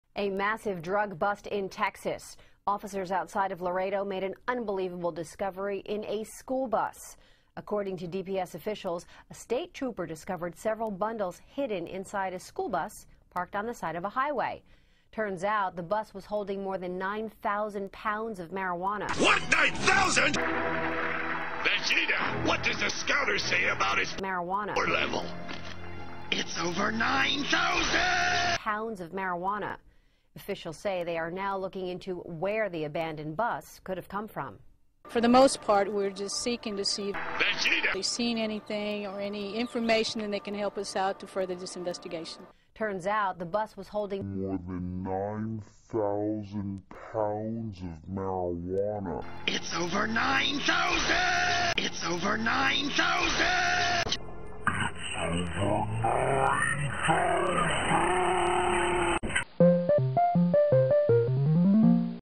OVER 9000!!!! POUNDS OF WEED
over 9000 lbs of weed found in school bus